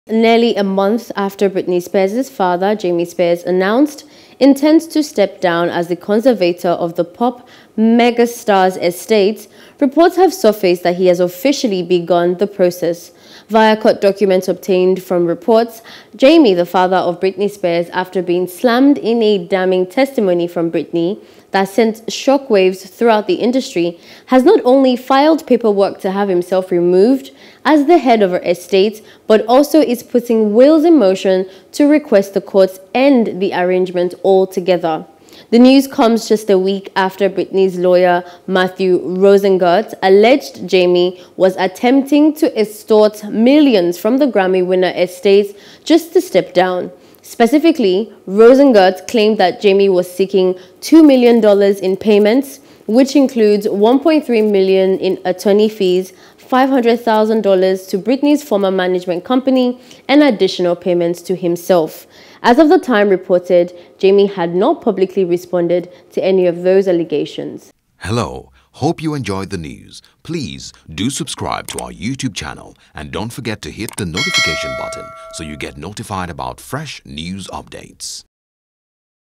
Britney Spears’ Father Files To End Conservatorship | ENTERTAINMENT
Nearly a month after Britney Spears ‘father, Jamie Spears, announced intent to step down as the conservator of the Pop megastar’s estate, reports have surfaced he has officially begun the process.